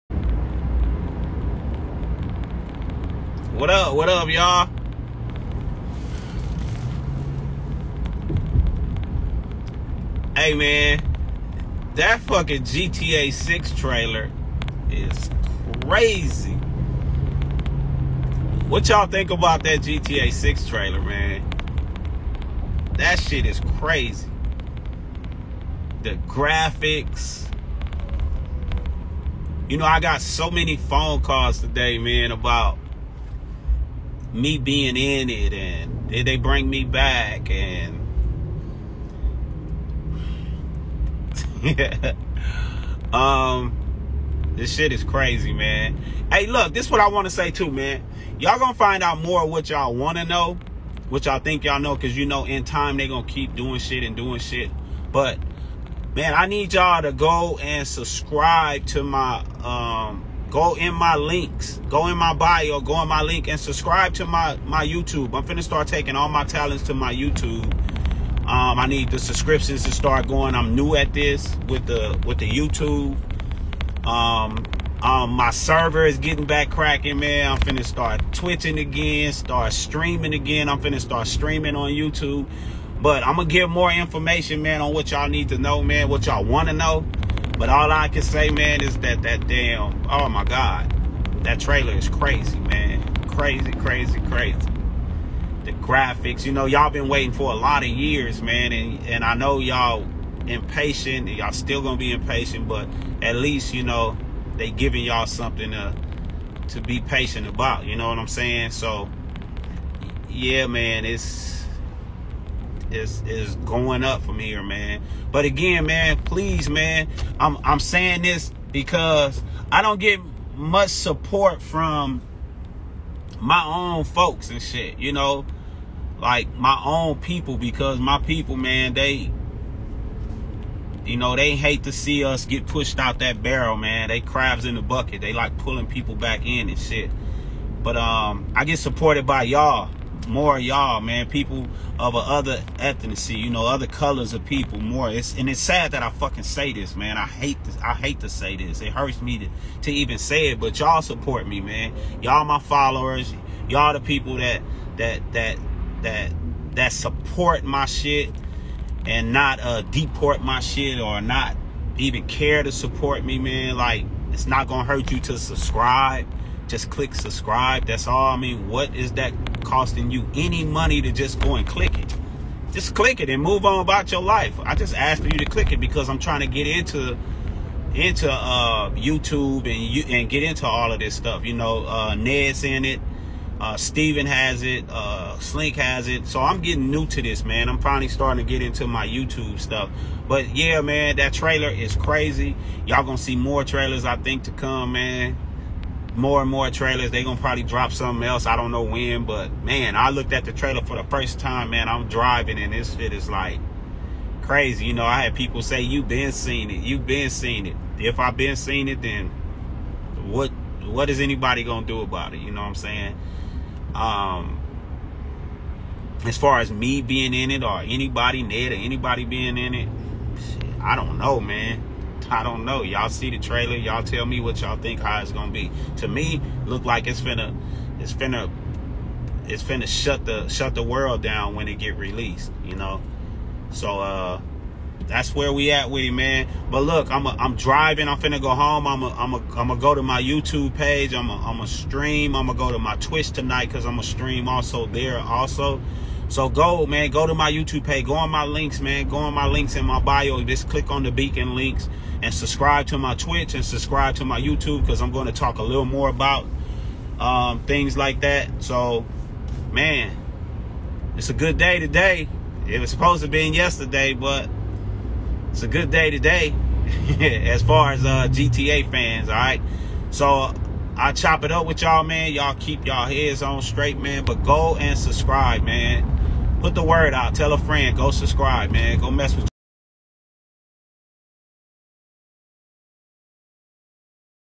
Shawn fonteno talks about gta 6
shawn fonteno talks about gta6 trailer
ps. I saw the trailer and I still can't believe my eyes